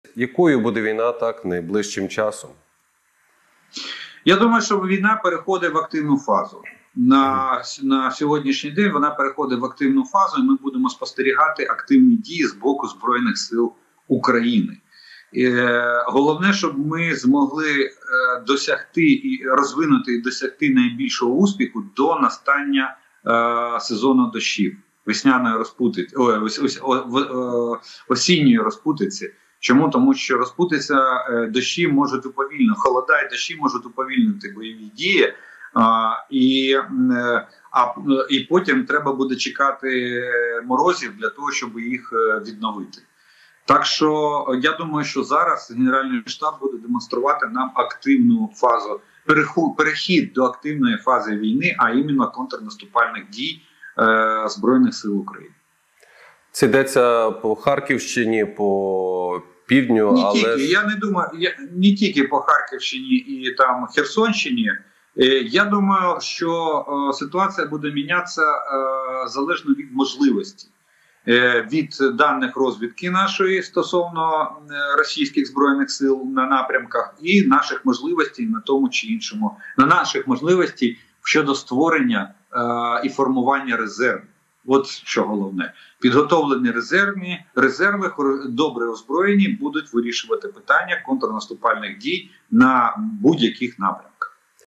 Чи вплинуть дощі на контрнаступ? Відповідь Жданова
Олег Жданов заявив, що  війна переходить у активну фазу. Водночас, він занепокоєний тим, щоб ЗСУ встигли досягти найбільшого успіху до початку дощів.

Ukraine.Media
Lviv.Media